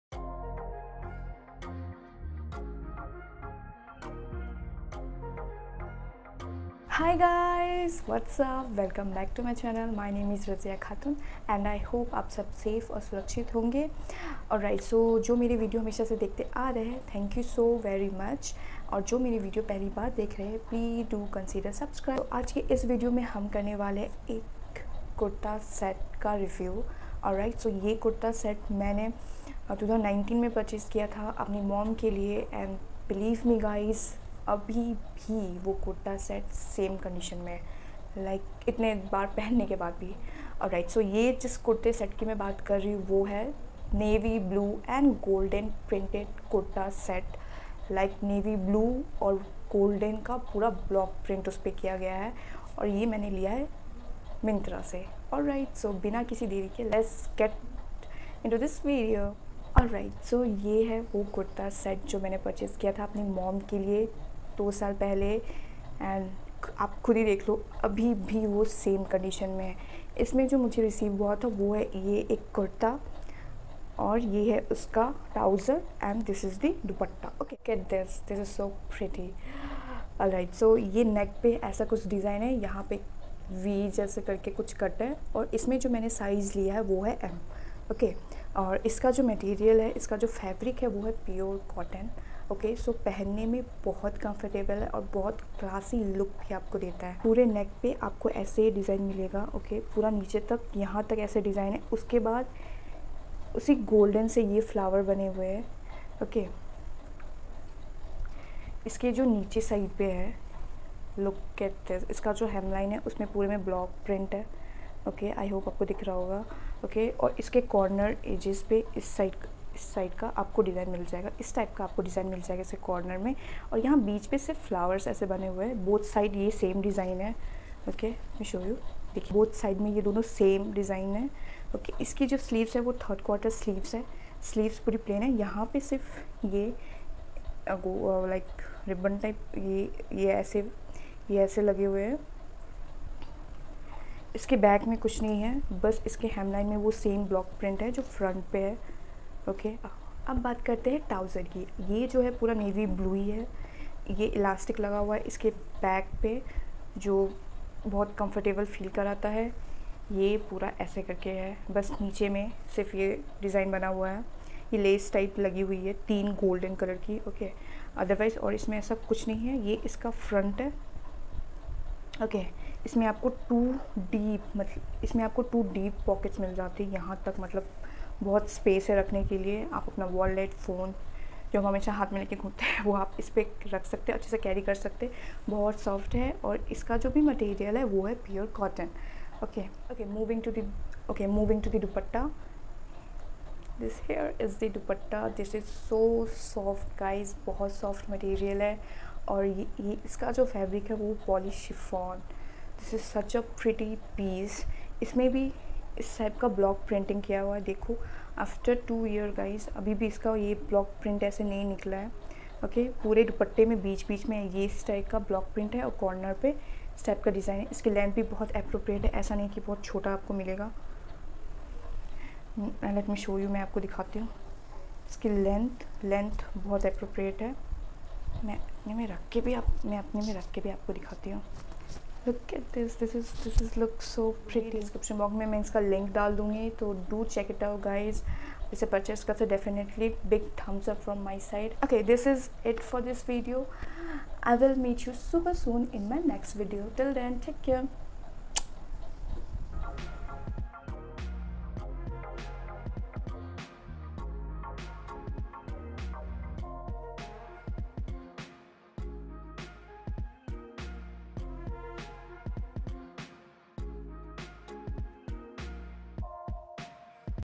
Libas Women Navy Blue & Golden Block Print Kurta set | purchase 2 year back |Myntra |@899 only
Hey GuyZZ!!

Check it out, this awesome kurta set from brand Libas. I purchase this two years back for my Mom and believe me its still the same conditions. So, i thought to share with you all.

Libas
Women Navy Blue & Golden Block Print Kurta with Trousers & Dupatta

PRODUCT DETAILS
Navy Blue and golden printed kurta with trousers and dupatta
Navy Blue and golden printed straight calf length kurta, has a round neck, three-quarter sleeves, side slits
Navy Blue solid trousers, has partially elasticated waistband, slip-on closure, two pockets
Navy Blue and golden printed dupatta, has printed border

Size & Fit
size M

Material & Care
Kurta fabric: Pure cotton
Bottom fabric: Pure cotton
Dupatta fabric: Poly chiffon